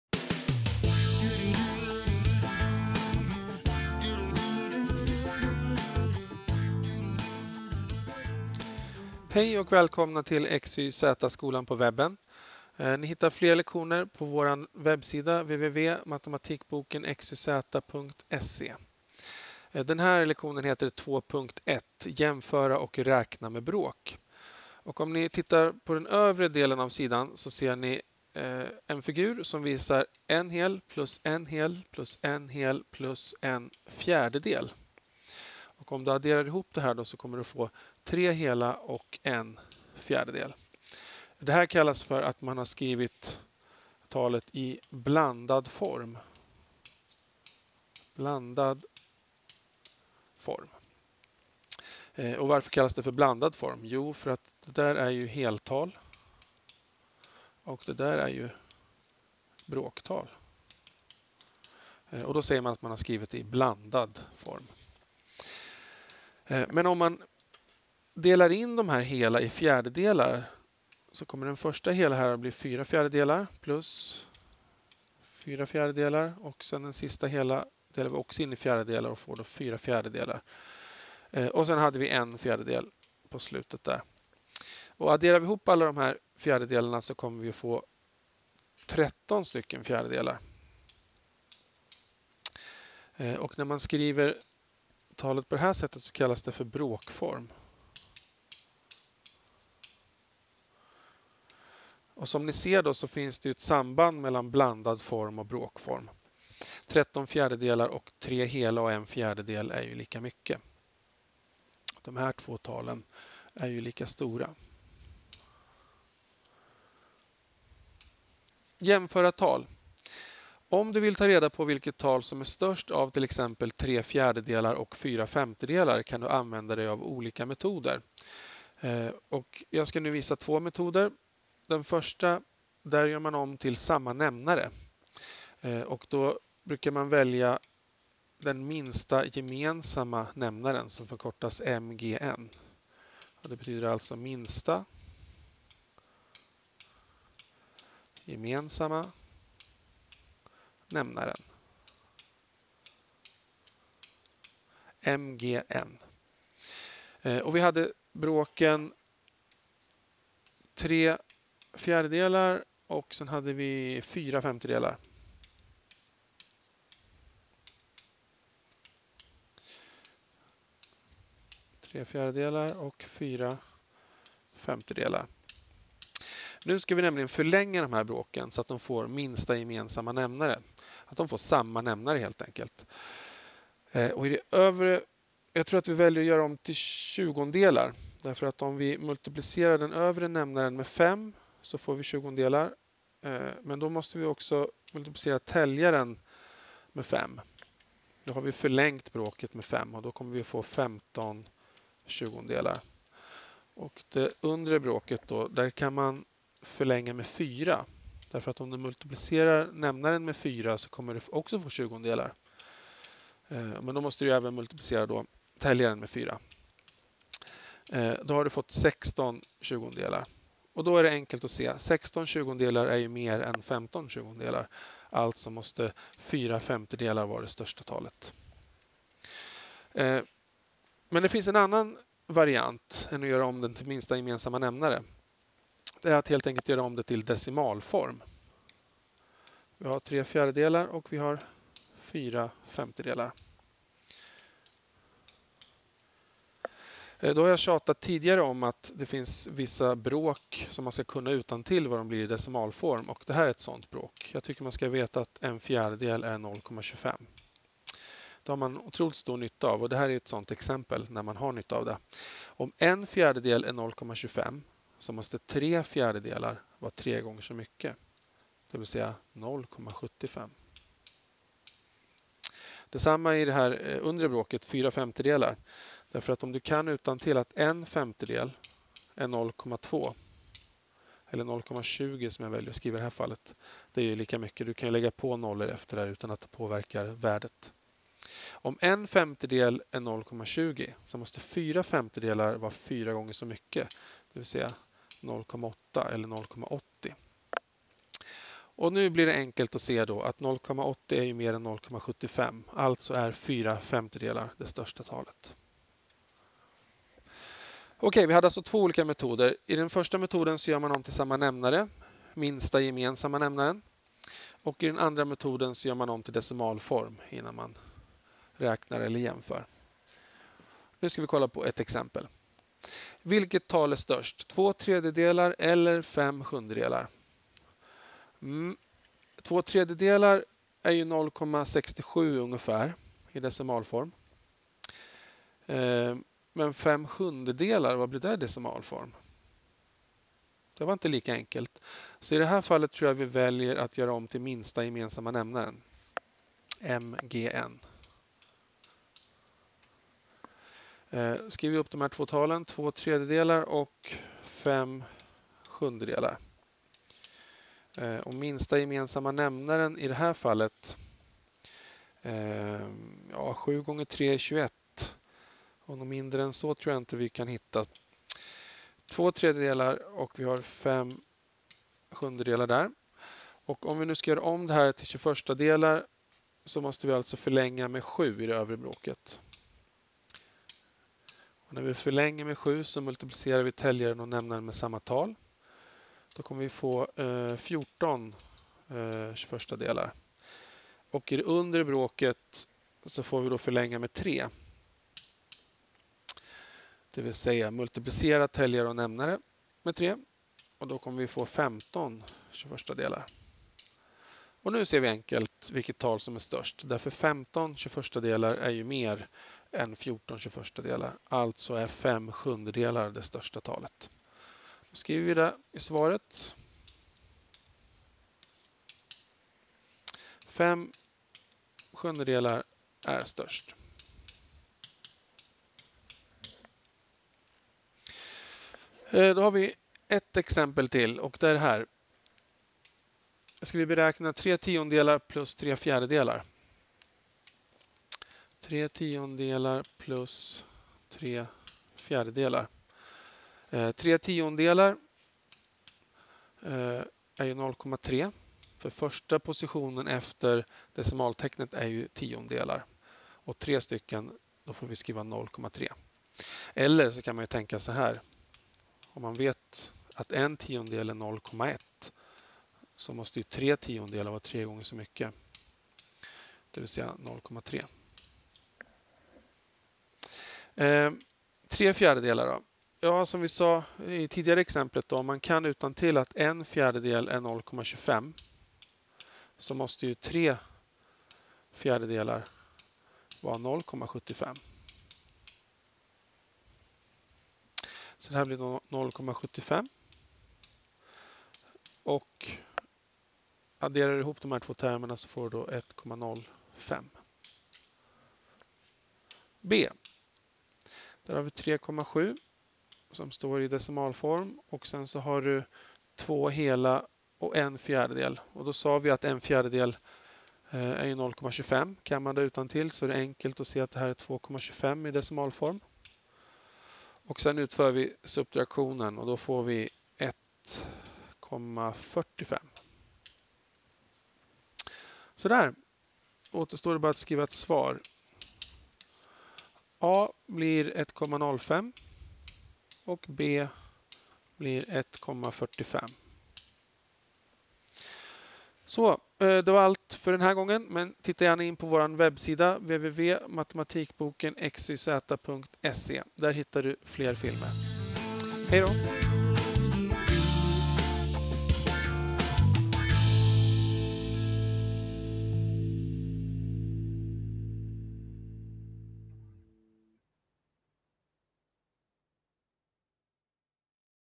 Y 2.1 Jämföra och räkna med bråk
I den här filmen får du lära dig sambandet mellan "blandad form" och "bråkform" samt hur man kan räkna med bråk genom att omvandla talen till bråktal med samma nämnare eller till decimaltal.